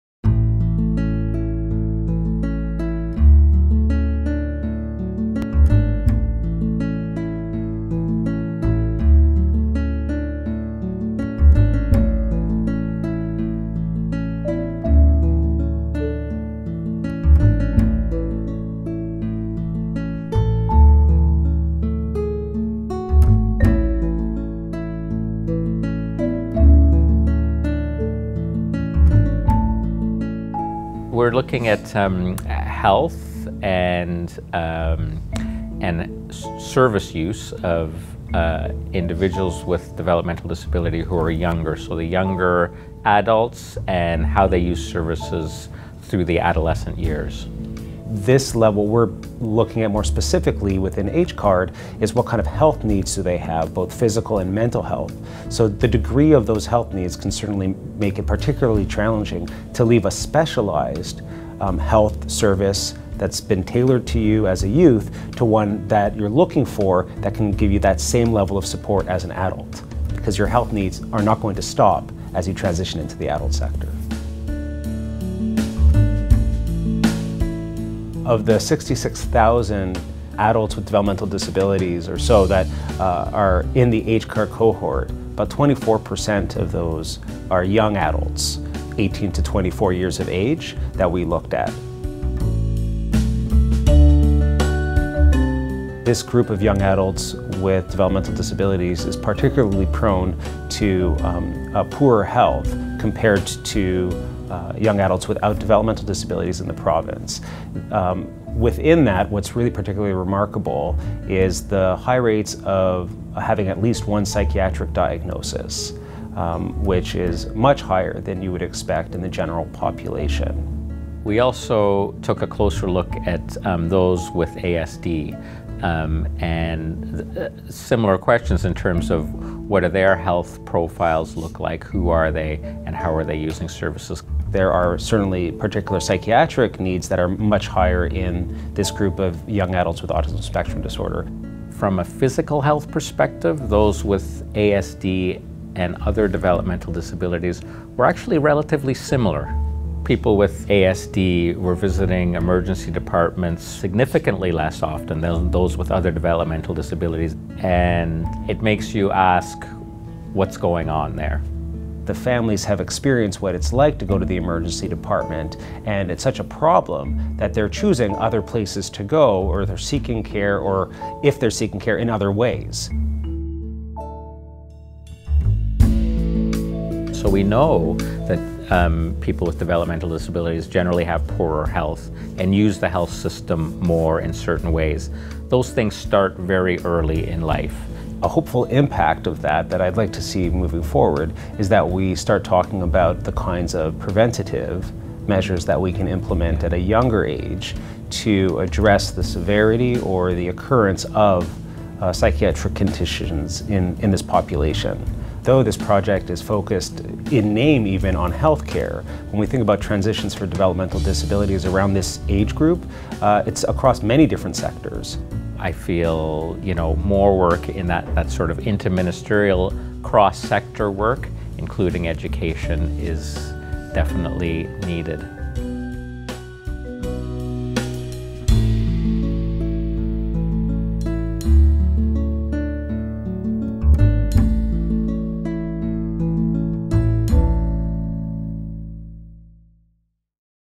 H-CARDD Transition Age Youth Project
Health Care Access Research and Developmental Disabilities (H-CARDD) is a research program that aims to enhance the overall health and wellbeing of individuals with developmental disabilities through improved health care policy and improved services.

Watch this video to see a brief overview of the Transition Age Youth project and findings. for more in depth information on all topics and projects.